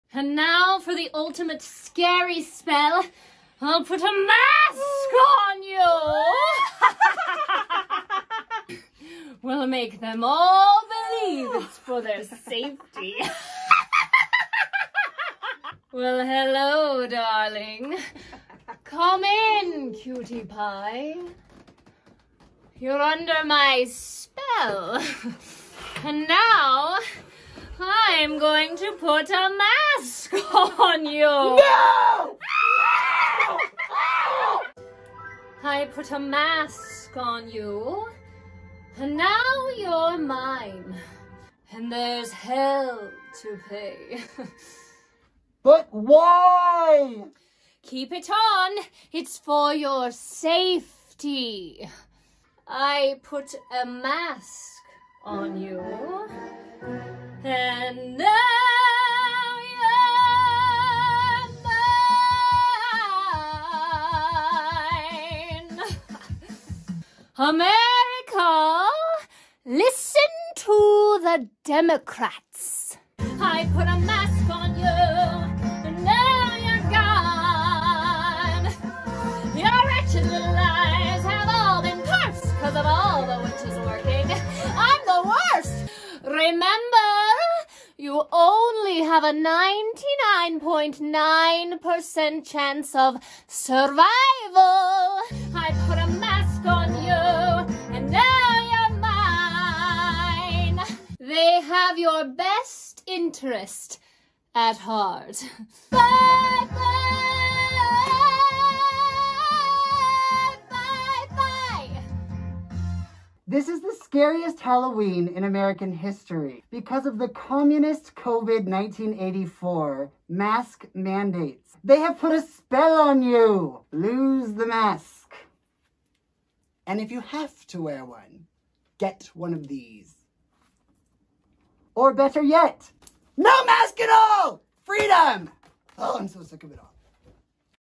I put a Mask on You!
THEY PUT A SPELL ON YOU!
This will be the scariest Halloween of all time.
Why?
Masks are psychological terror.
We’re muzzled.
We’re dehumanized.
Doctors are silenced for questioning the narrative.
In the name of “safety” we can lose everything.
The words “for your safety” have literally ERASED freedoms across the globe.
Why are people not standing up?
I’m so tired of watching people walking around like broken, sad sheep.
If you’re vulnerable, by all means—wear a mask!
But for the rest of us, especially these poor children everywhere, it’s EVIL.
Masks first, lockdowns were next, gatherings prohibited, full government control, mandatory tracking, family separation...what’s next?
Listen to bidenharris2020 and you’ll know.
America: You failed their psychological test. Their experiment worked.
Shame on us.
We must save our country and vote for @realdonaldtrump